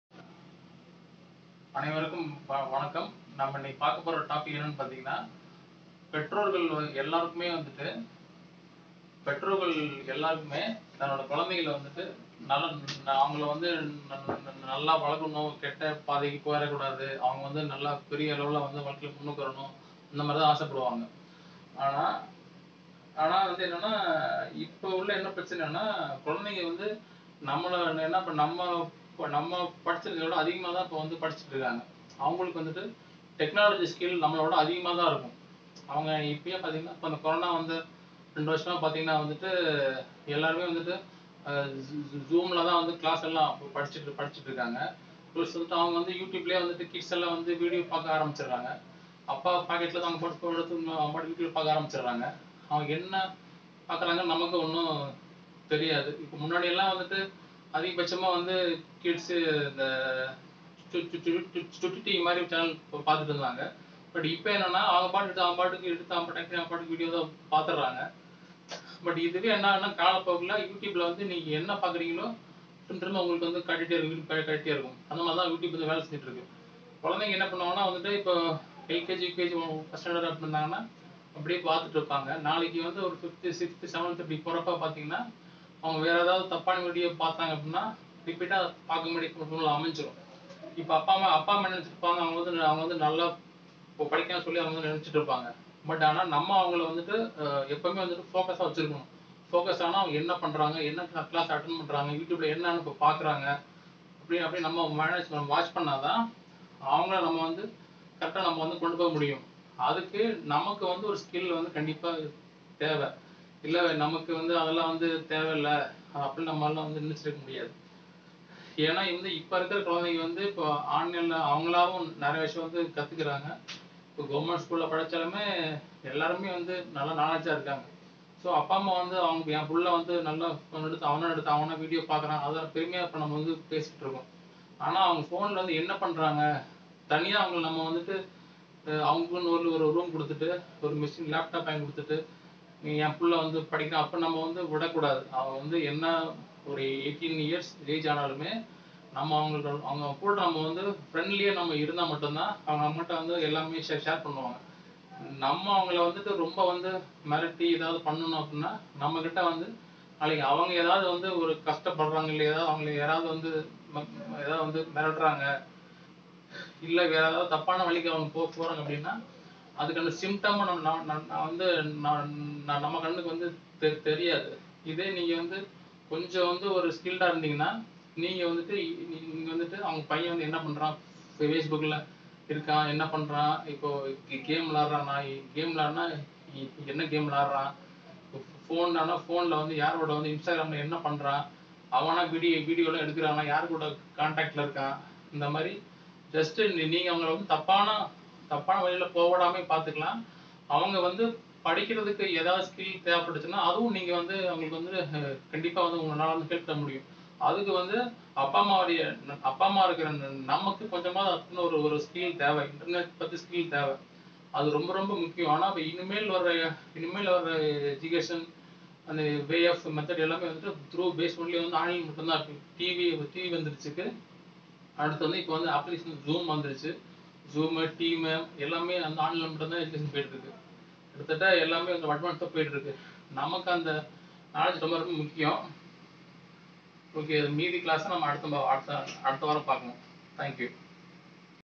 Why Technology skill is important for Parents?
நாம் குழந்தைகளை சரியான முறையில் வளர்க்க தொழில்நுட்பம் பற்றிய அறிவு மிகவும் அவசியம்,அது ஏன் என்று இந்த வீடியோவில் பார்க்கலாம்.